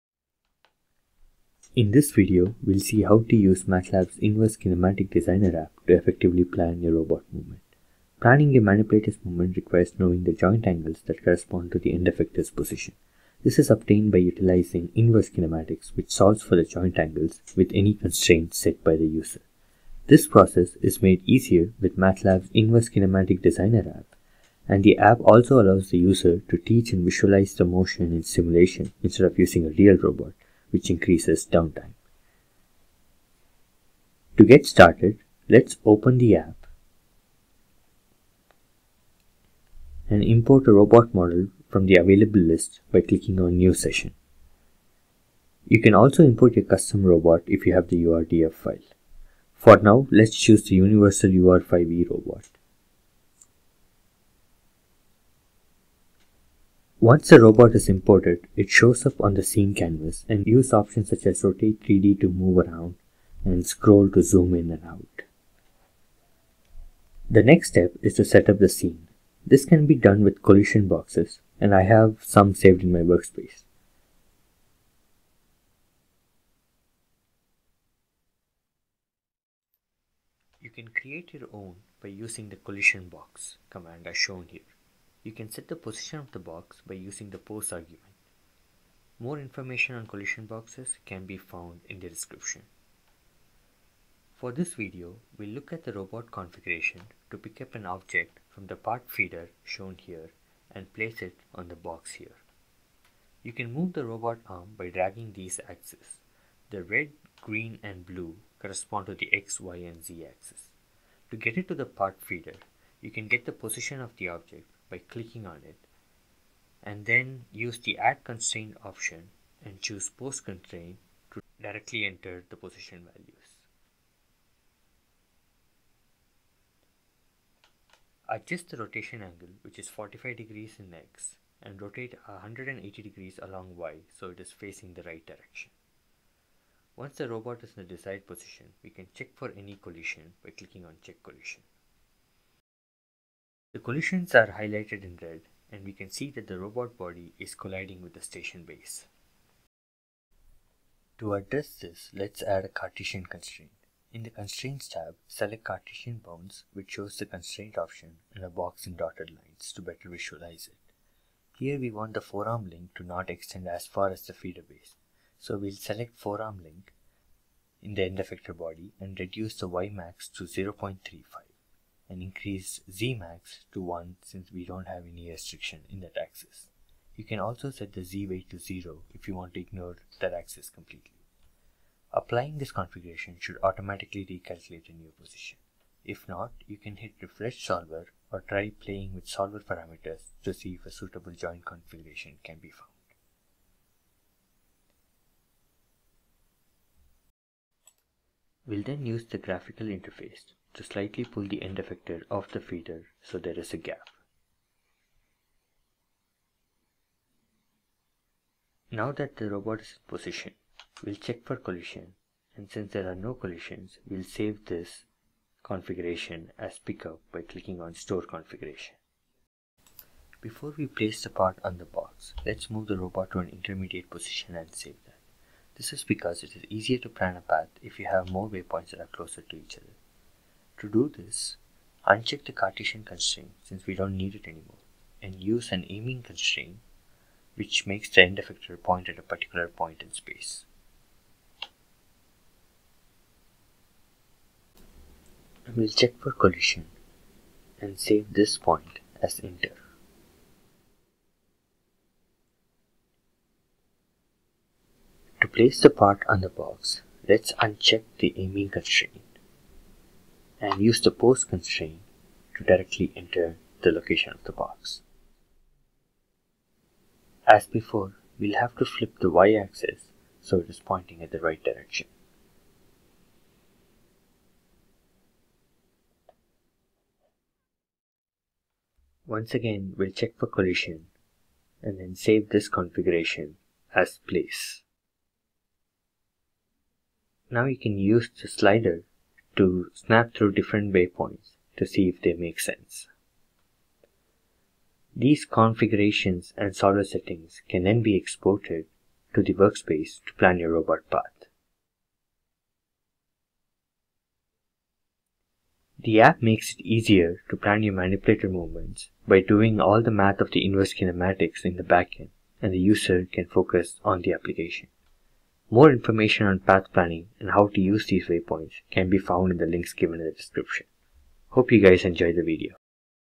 How to Plan Your Manipulator Movement
Learn how you can use the Inverse Kinematics Designer app in MATLAB® to plan your robot manipulator movement. Planning your robot’s movement with hardware can be challenging and time-consuming. This demonstration goes over how you can import a robot, set up the scene, set constraints for the IK solver, and export the waypoints for further planning, all in simulation.